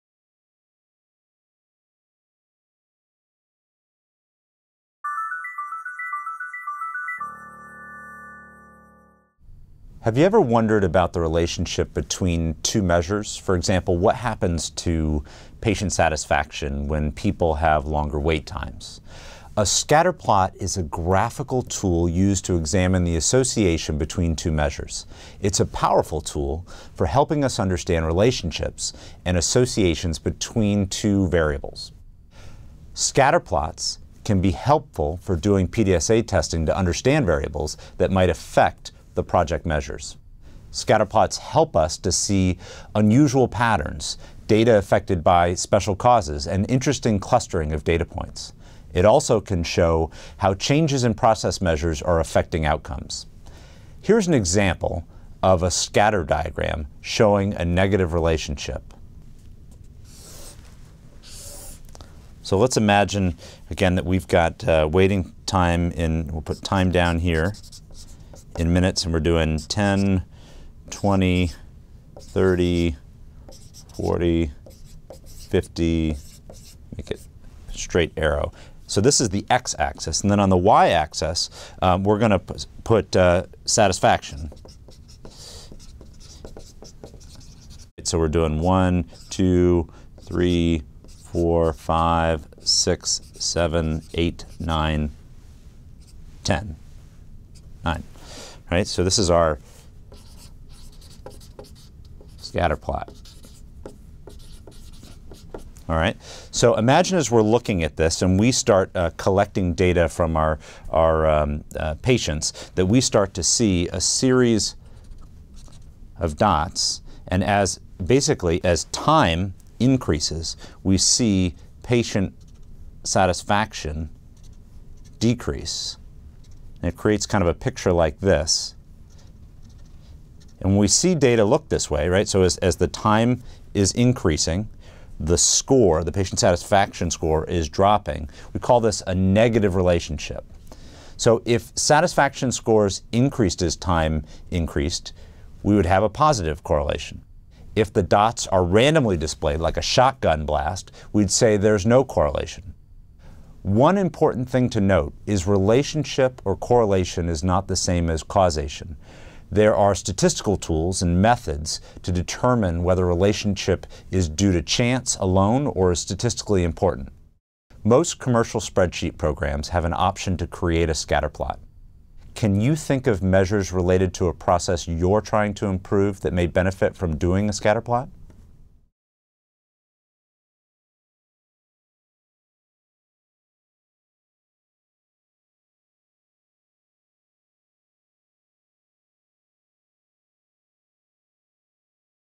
How to use scatter plots
From our free online course, “Practical Improvement Science in Health Care: A Roadmap for Getting Results”:

A scatter plot is a graphical tool used to examine the association between two variables. Scatter plots help us to see unusual patterns, data affected by special causes, and interesting clustering of data points. A scatter plot can also show how changes in process measures are affecting outcomes. Use a scatterplot when you want to determine the relationship between two different measures.